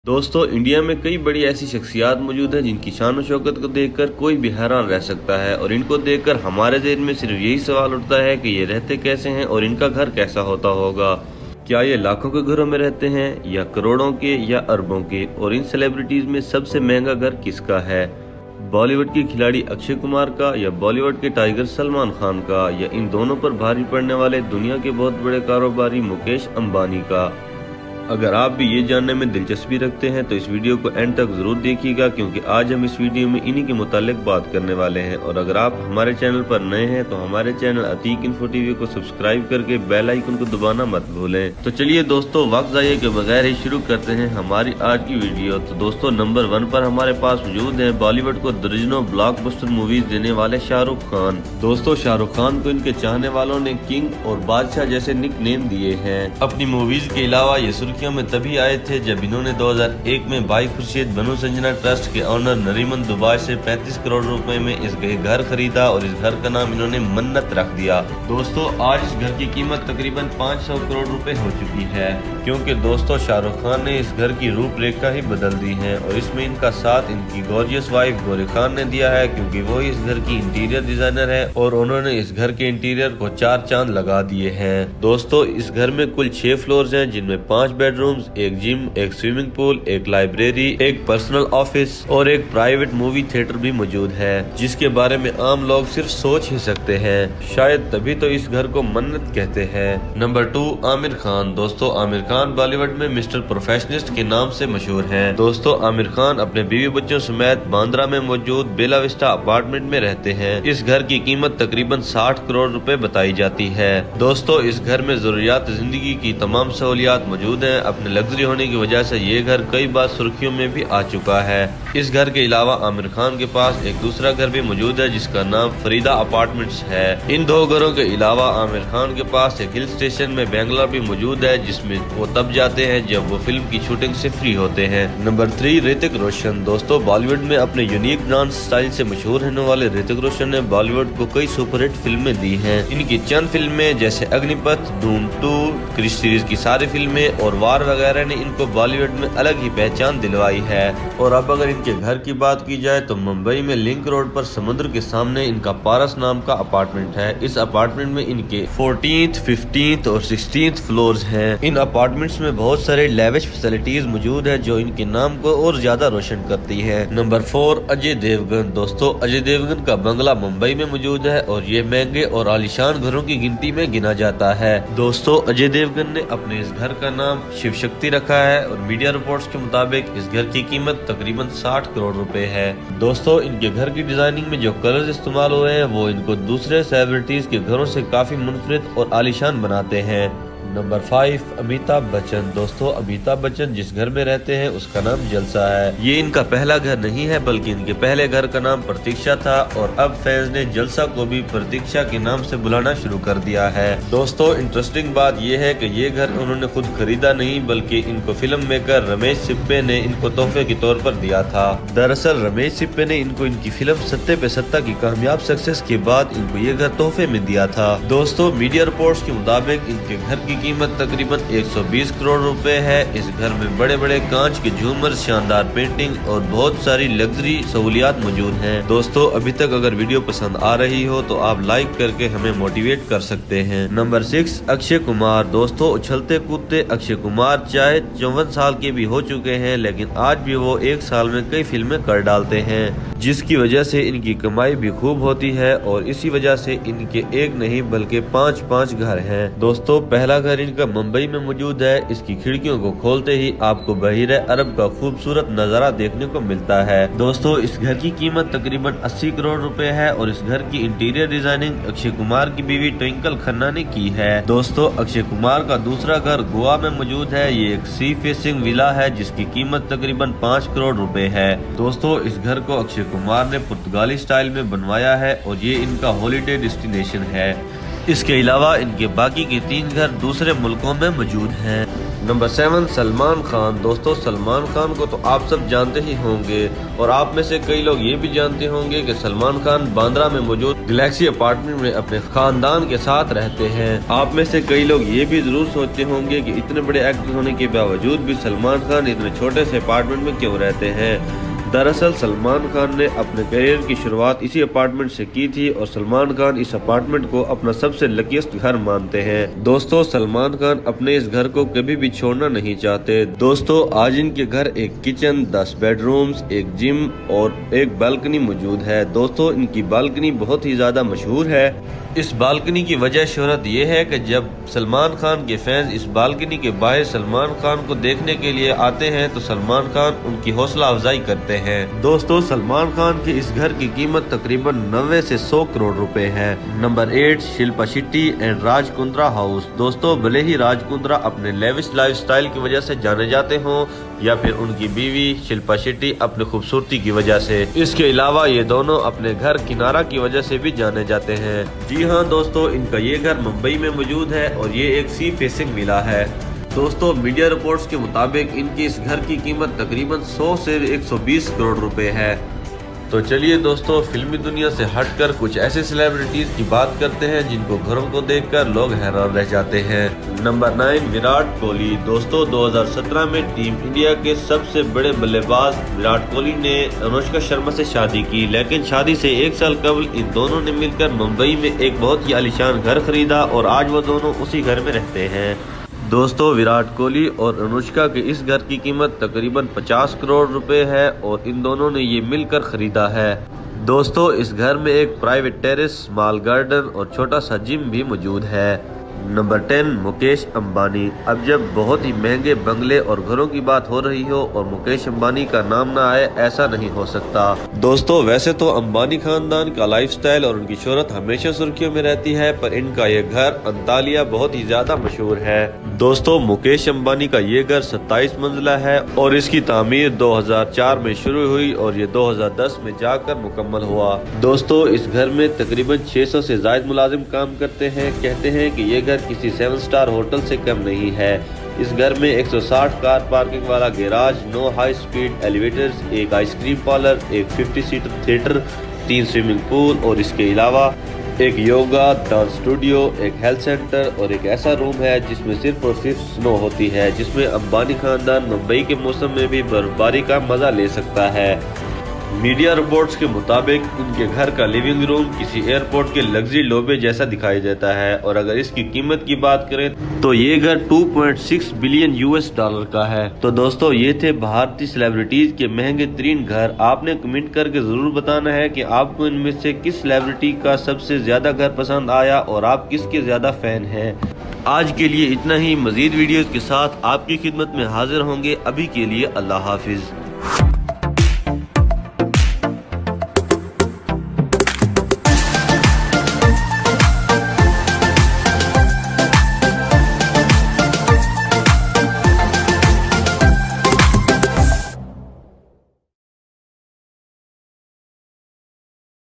Top10 Indian celebrities most expensive and luxury houses salmankhan,shahrukh,virat kohli|Urdu/hindi
00:00intro
00:48shahrukh khan house
01:44amir khan house
02:19hrithik roshan house
02:51ajay devgan house
03:14amitabhachan house
03:57akshay kumar house
04:46salman khan house
05:40shilpa shetty house
06:14virat kohli house
06:43muqesh ambani house

Friends in this we are going to discuss the top 10 most expensive and luxury houses of Indian celebrities.

We used a different footage, images, and clips in order to define the topic more clearly.

More from Ateeqinfotv